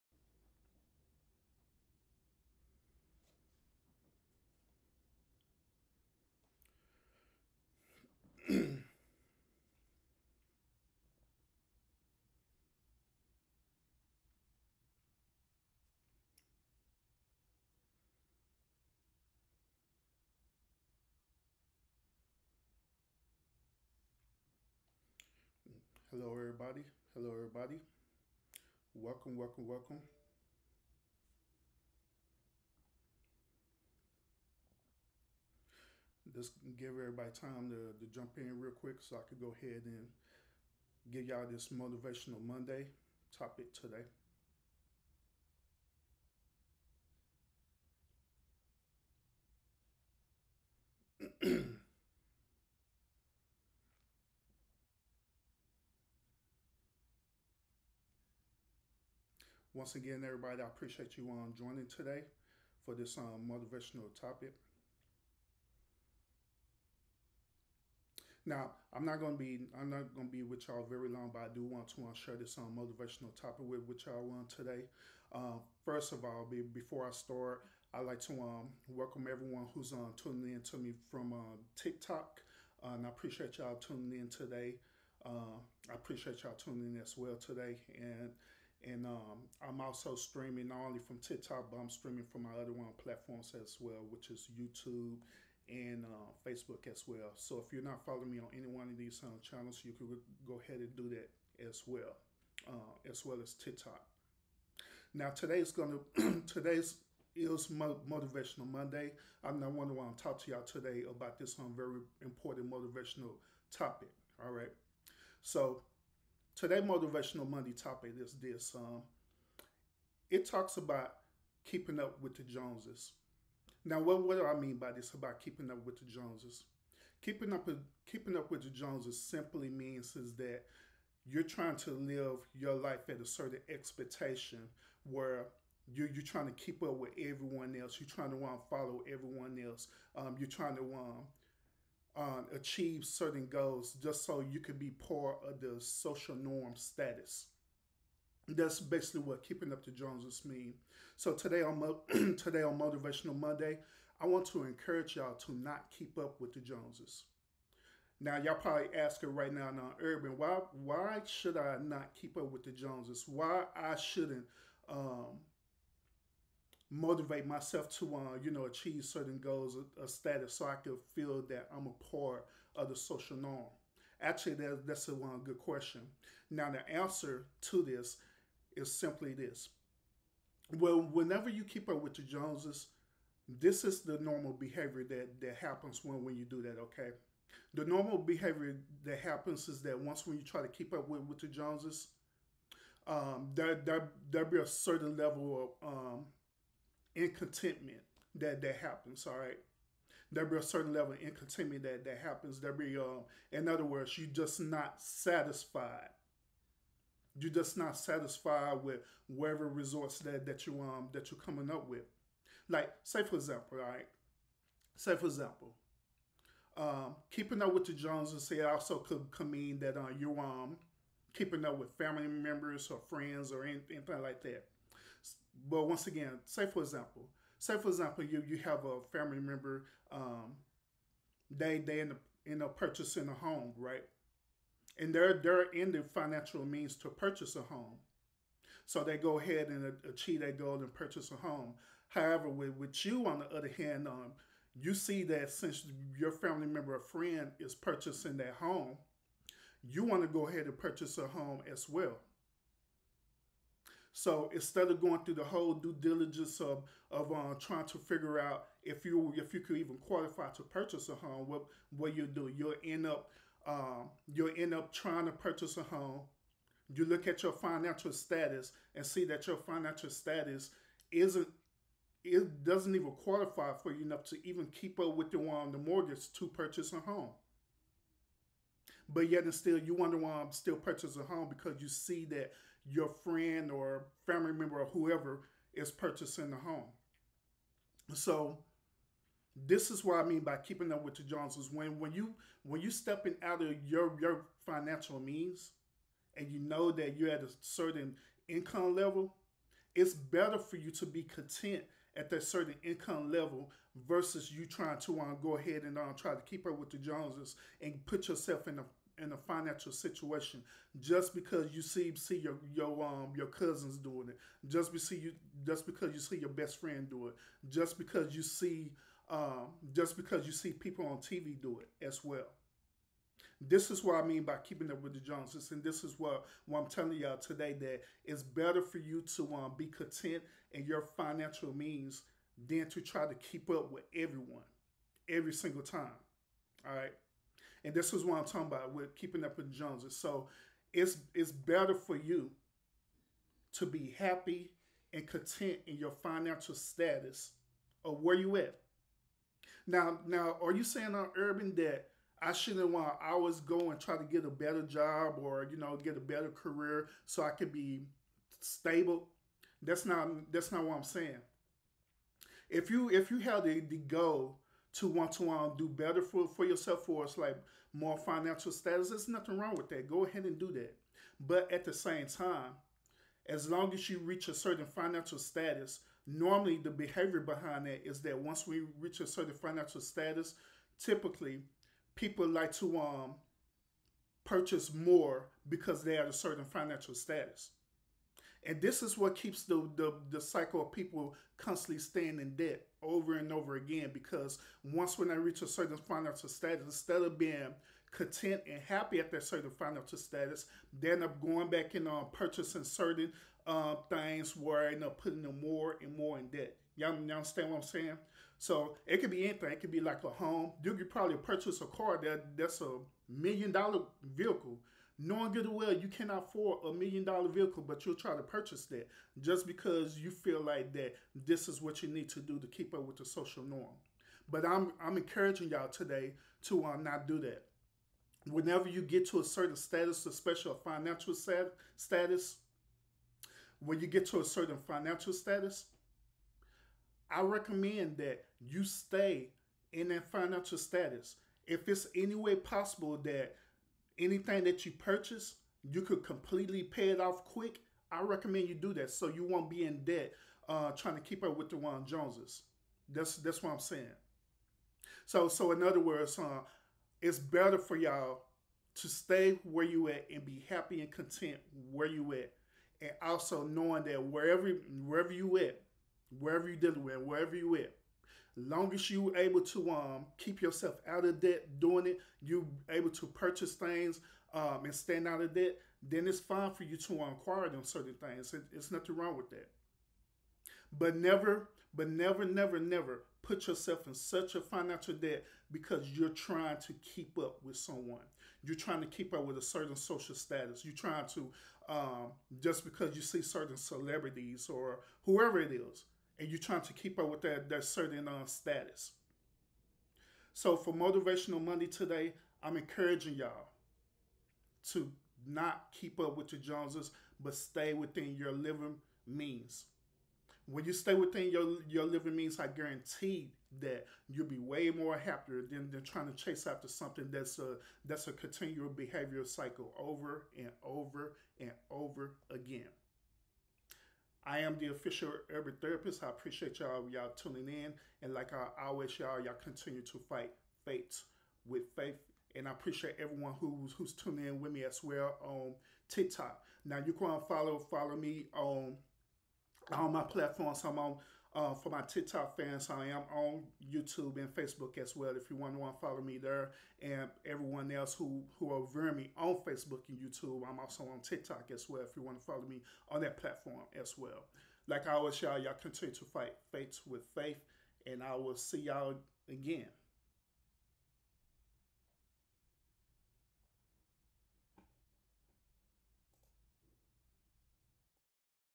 Motivational Monday: Don't Keep Up With The Joneses!
Today motivational topic focus on the importance of not living beyond your means just to keep up with social norms.